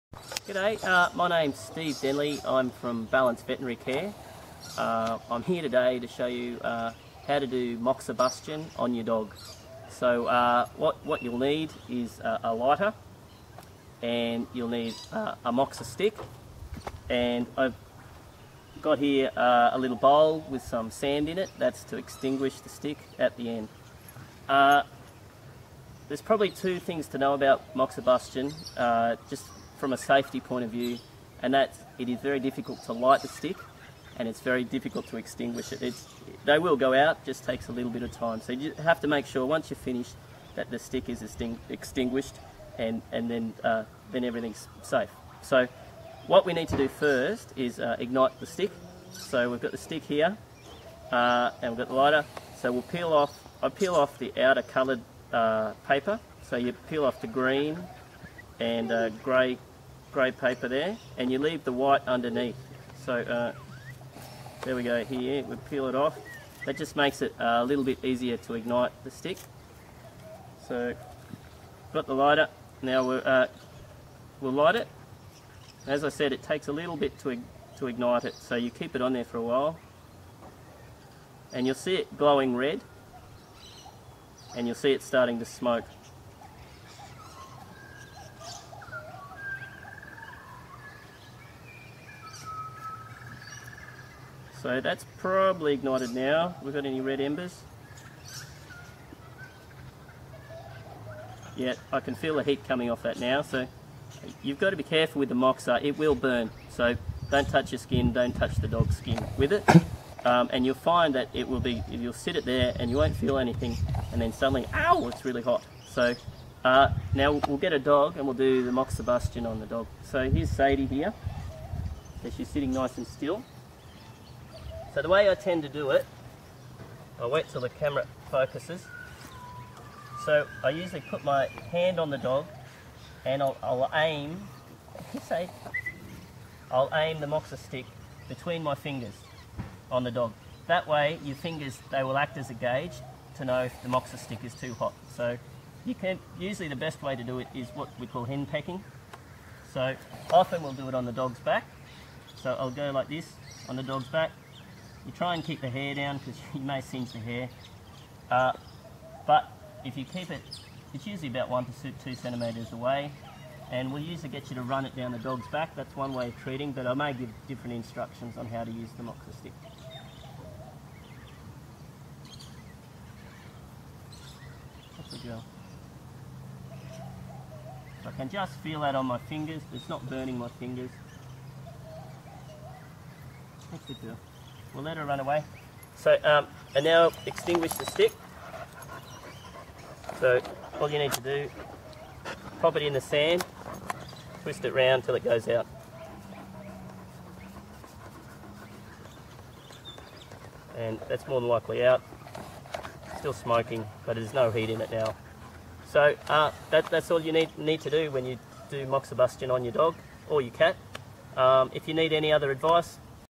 Moxibustion for your pet
Dr Steve Denley BVSc CVA shows you how to perform moxibustion on your pet.